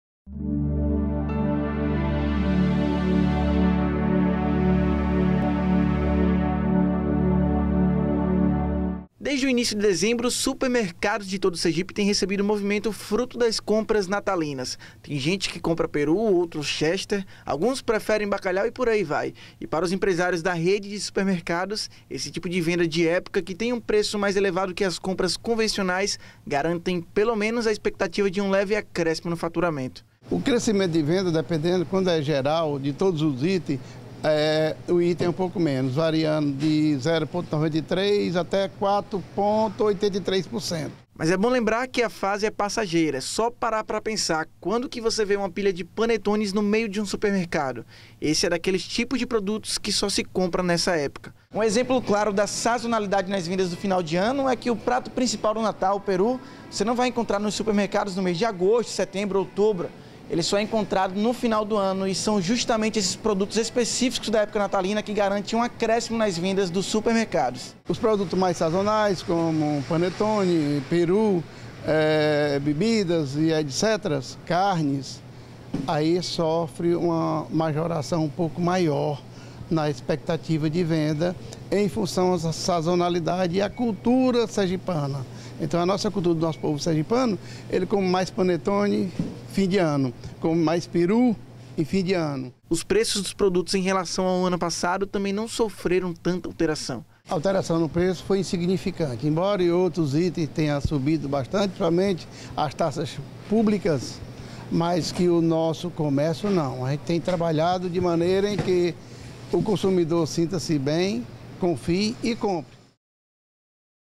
Compras natalinas aumentam vendas em supermercados
As compras natalinas em supermercados em 2017 deve superar a do mesmo período no ano passado, mas em número bastante tímido. Na análise de venda geral da Associação Sergipana de Supermercados, o percentual de crescimento deve variar de 0,9 % a 4,8%. O aumento das vendas é provocado, principalmente, pela procura por produtos sazonais como peru e chester